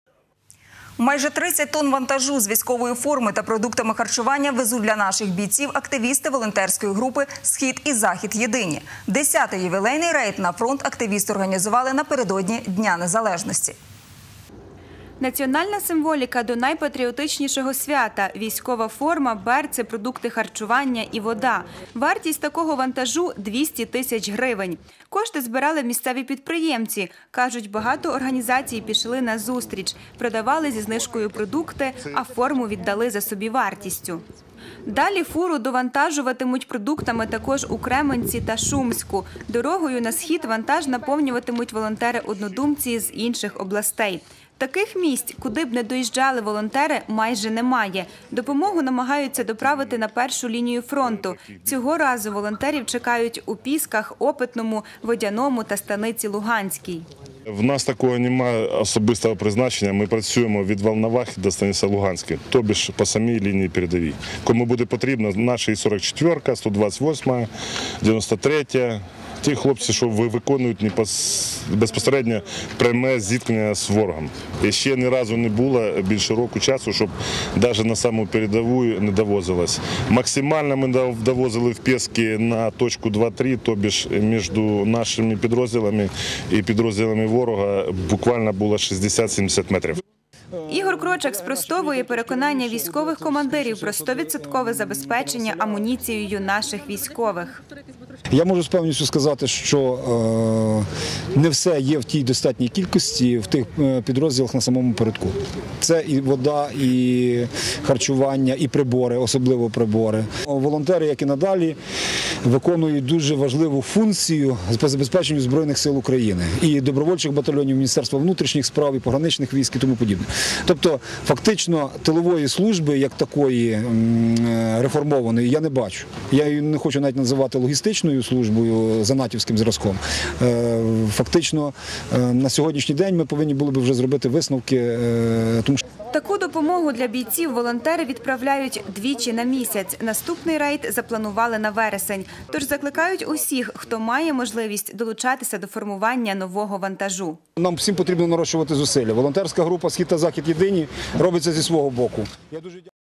Тернопільські активісти повезли 30 тонн допомоги бійцям АТО
Майже 30 тонн вантажу з військовою формою та продуктами харчування везуть для наших бійців активісти волонтерської групи «Схід і Захід єдині». 10-ий «ювілейний» рейд на фронт активісти організували напередодні Дня Незалежності.